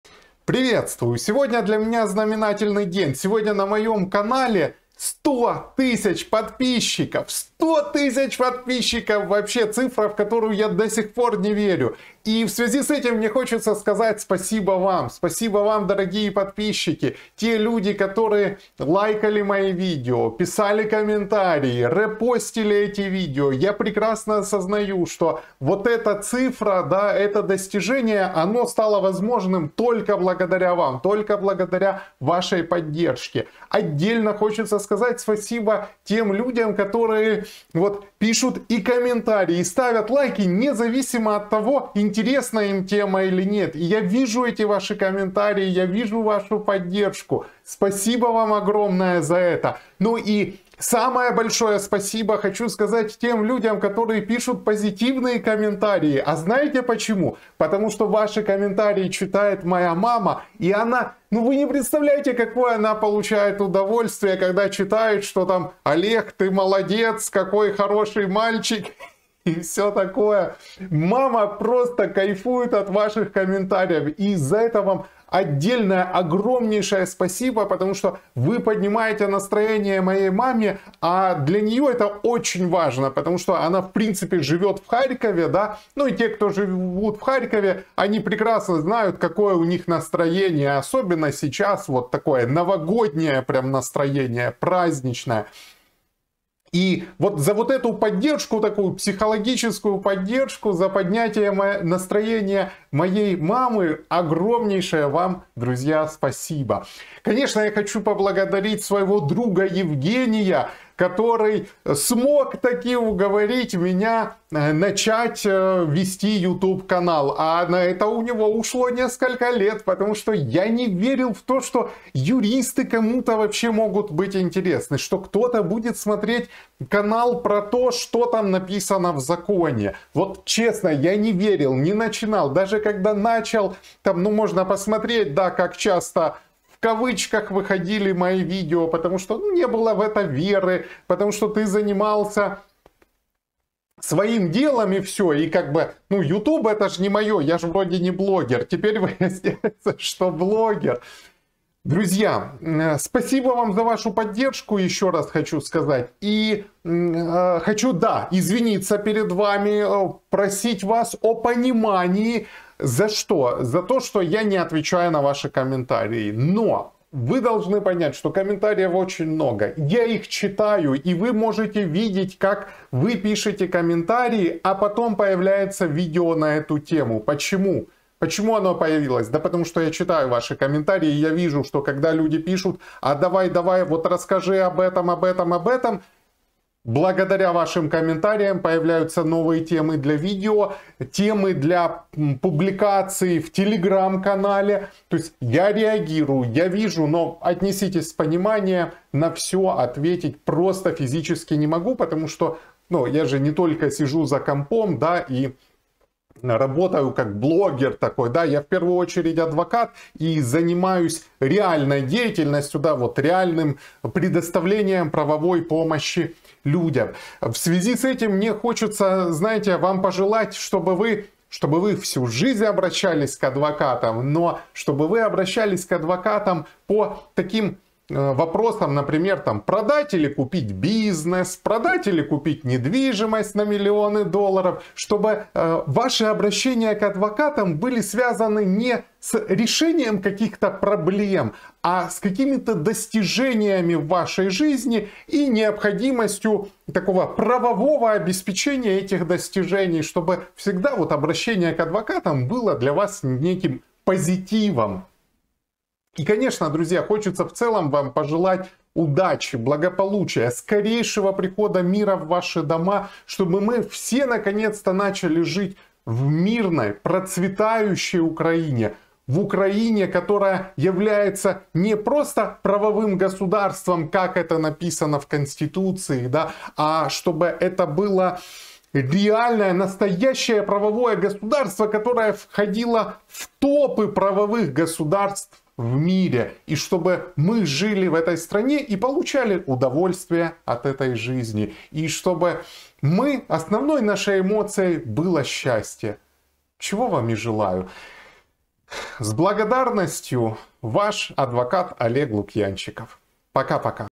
100 000 подписчиков на канале Адвокат Олег Лукьянчиков
В этот день хочу поделиться позитивными эмоциями и сказать спасибо всем, кто лайкает, комментирует и репостит мои видео. Именно благодаря вам это стало возможно. Отдельное спасибо моему другу Евгению, который уговорил меня вести юридический блог и рассказывать людям о их правах.